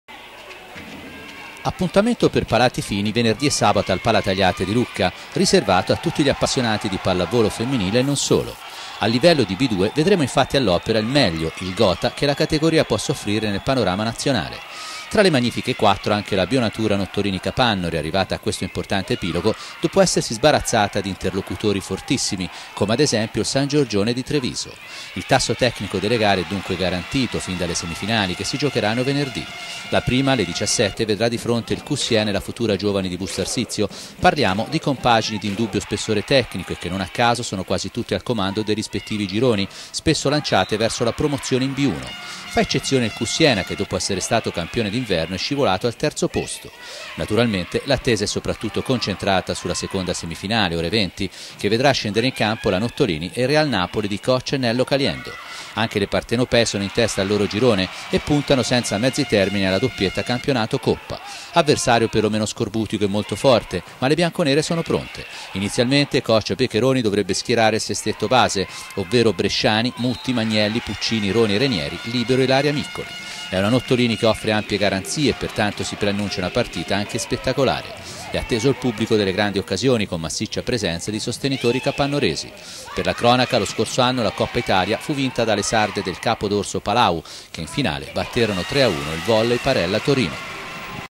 Anche la Nottolini Capannori ha il suo... Real
PALLAVOLO B2 DONNE - Si disputa venerdì e sabato al Palatagliate di Lucca la final four di Coppa Italia. In lizza anche la Nottolini Capannori che in semifinale se la vedrà con la compagine partenopea del Real Napoli. Nell'altra semifinale saranno di fronte Cus Siena e Busto Arsizio.---

NoiTv